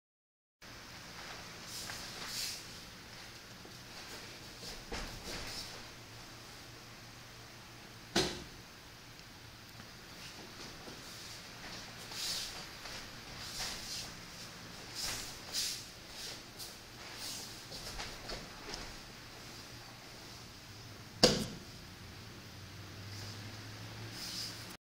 Katatori Ikkyo Tenkan
7th kyu test requirement Shoseikan Dojo, Frederick, MD